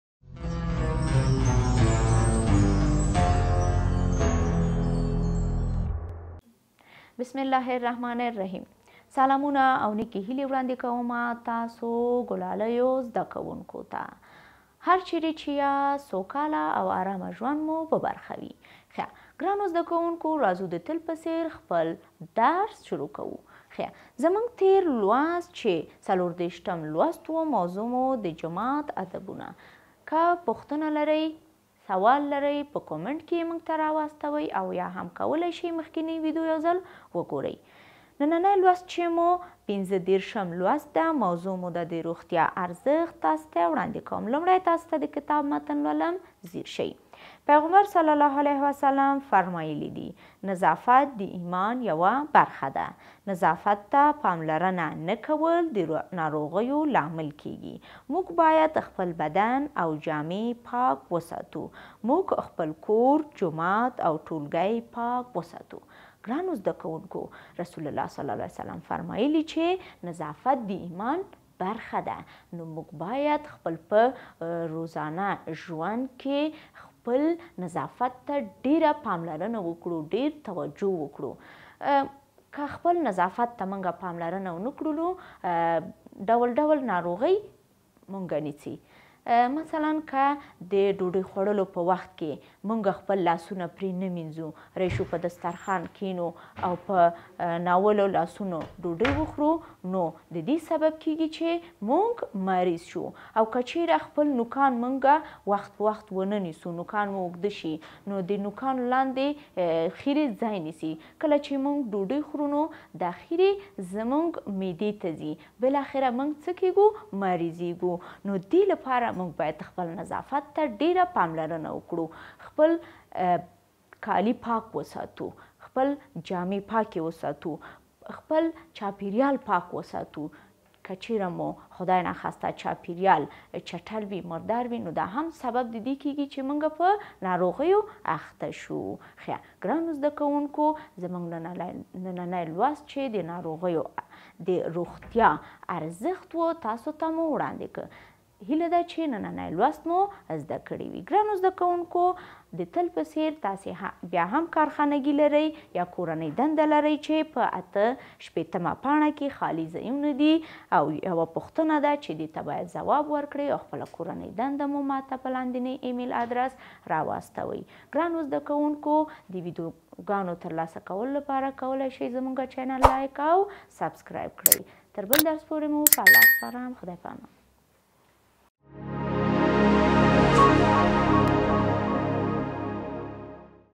Class 2 - Islamic Subject | The value of health | Do sport in order to be healthy.
In this lesson you will learn about the health value and the way to praise for Allah.

The value of health
د روغتیا ارزشت
ارزش سلامتی

Class 2  - LESSON 35  - ISLAMIC SUBJECT  / ۳۵ دویم ټولګی - د اسلامی ښونې او روزنې مضمون  -   لوست

د صبار_افغان_آنلاین_ښونځی